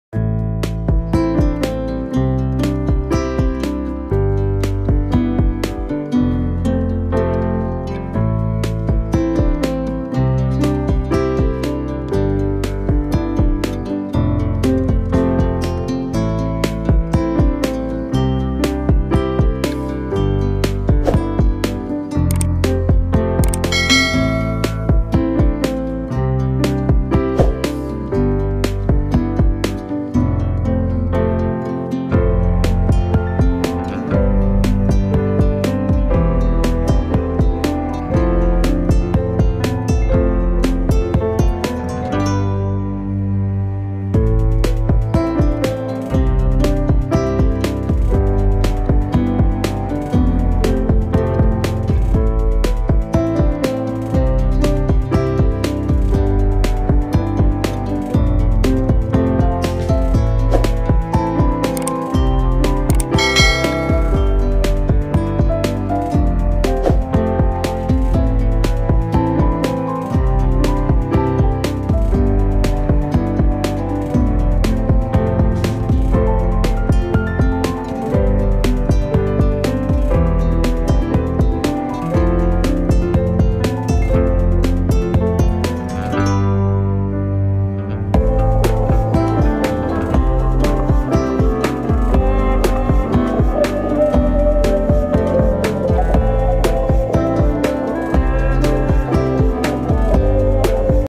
Find 221 / In 7 Seconds 😊
Here are 7 quizzes designed.
You have to find the number 221 among the numbers.